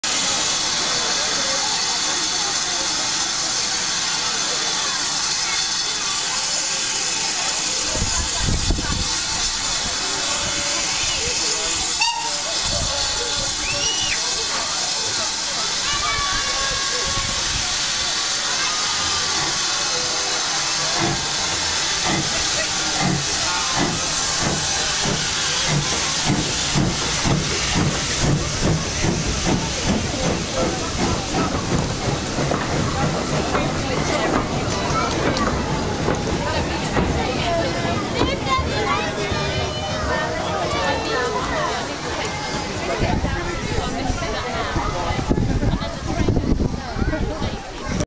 Thomas Day Out 2006
Thomas the tank engine day out with the kids, Watercress line UK